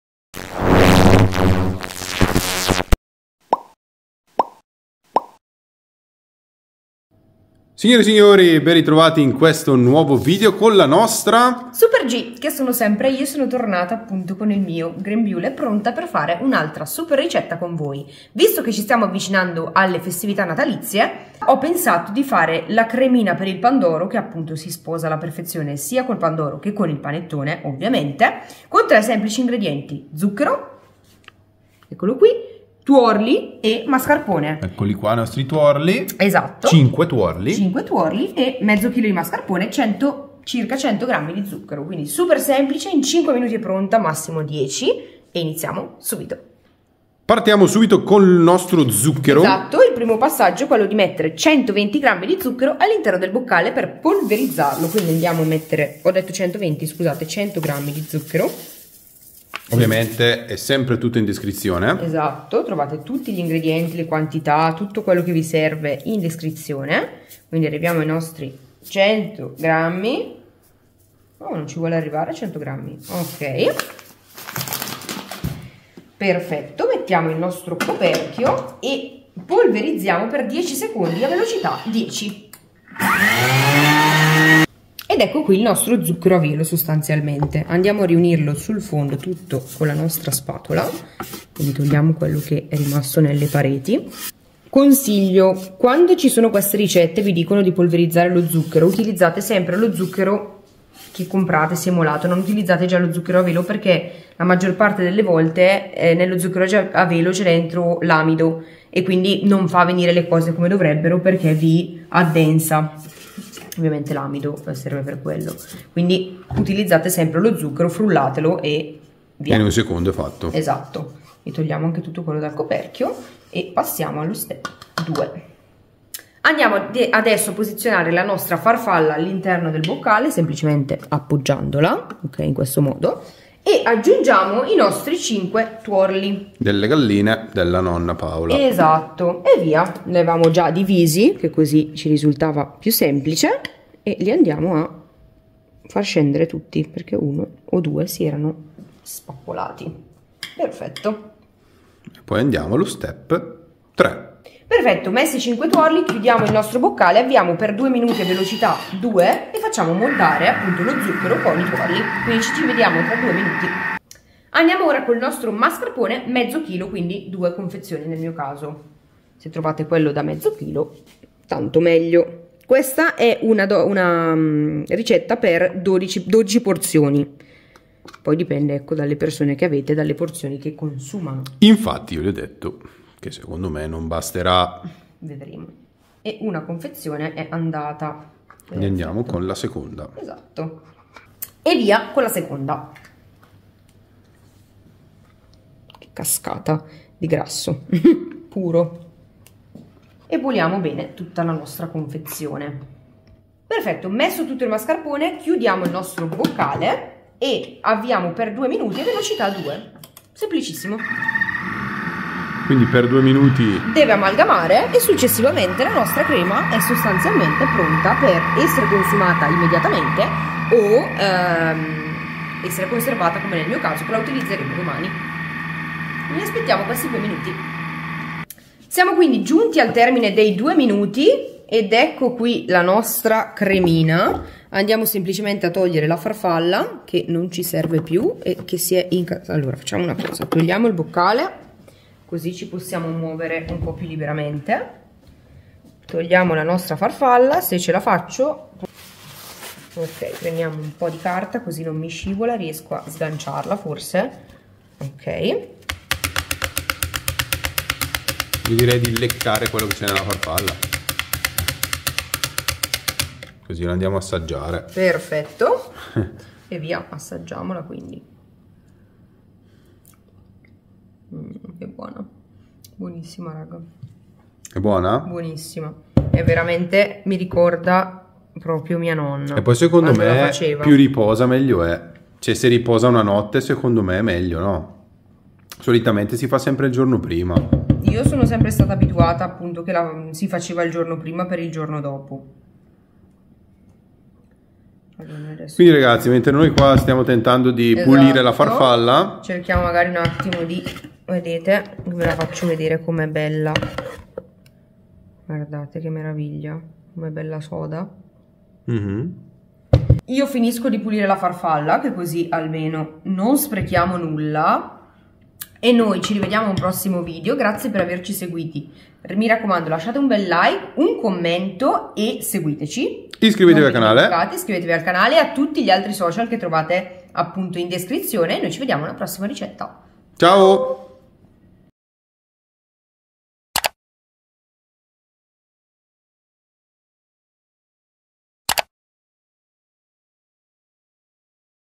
Crema Mascarpone per Pandoro | Ricetta Bimby Facile & Veloce | In Cucina Con SuperG
Ingredienti Crema al Mascarpone per Pandoro 👇

120 g di zucchero
5 tuorli
500 g di mascarpone